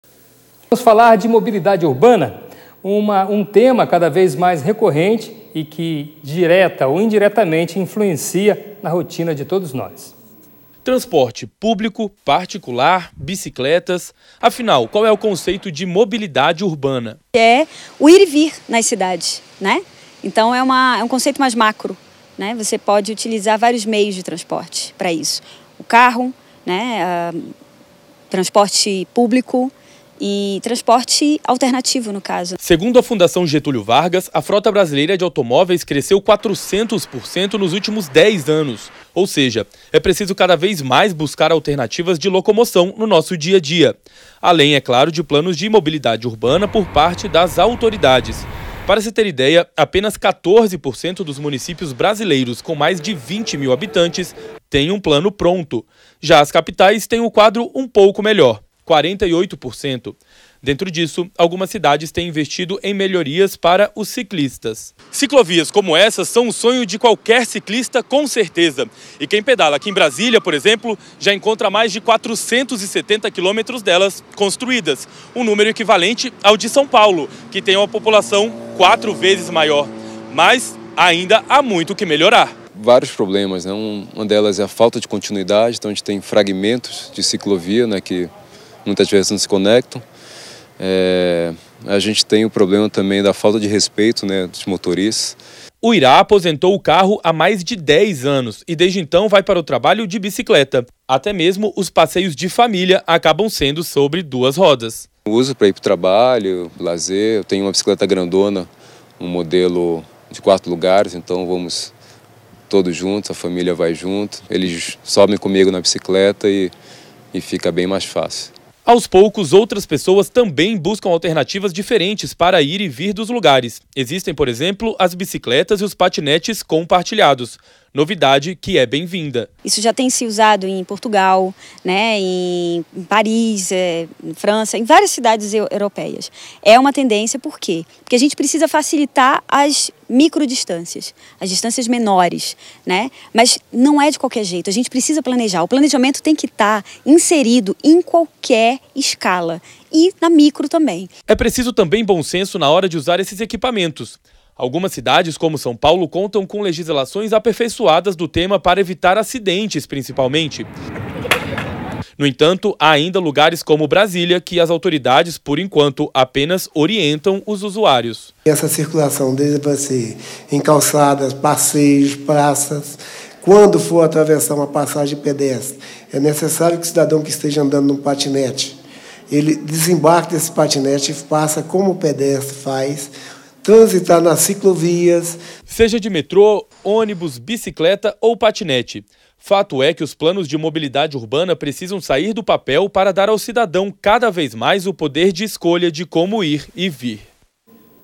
Mobilidade Urbana: tema que influencia cada vez mais a rotina das pessoas
Mobilidade urbana, um tema cada vez mais recorrente e que, direta ou indiretamente, influencia na rotina de todos nós. E os desafios da mobilidade urbana no Brasil é assunto do programa Além da Notícia. O programa vai ao ar, logo mais às dez da noite e terá a participação de convidados especiais para debater o tema.

Reportagem de Filipy Barcelos e Bartolomeu Silva